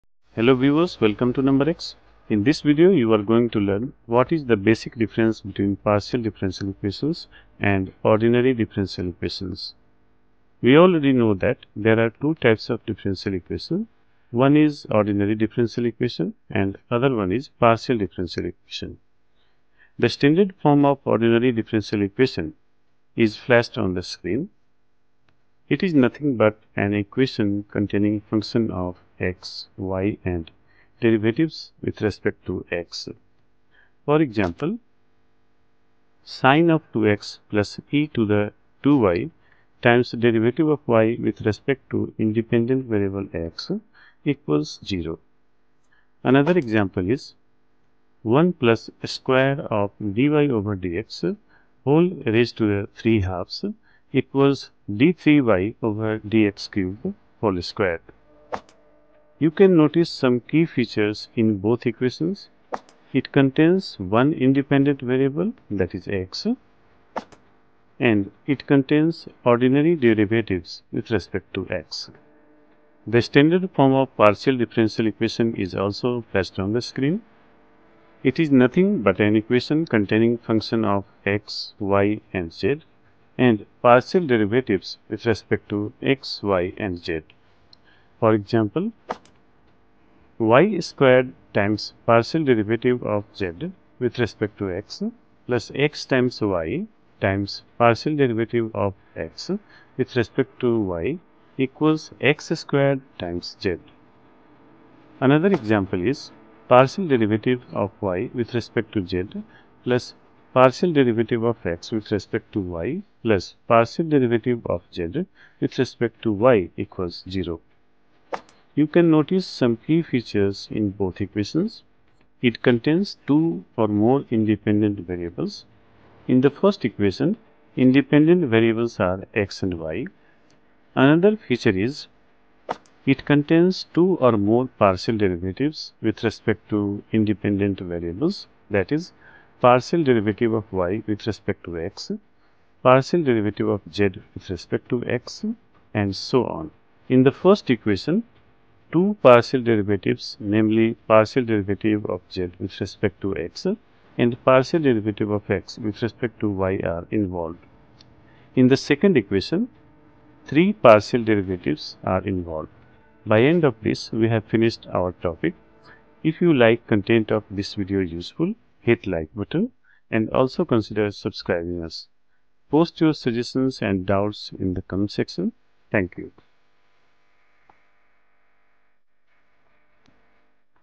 Partial Differential Equations and Ordinary Differential Equation Difference, Example | PDE ODE
0:00 Introduction.
0:19 Ordinary Differential Equations (ODE) Example, Definition.
1:13 Partial Differential Equations (PDE) Example, Definition.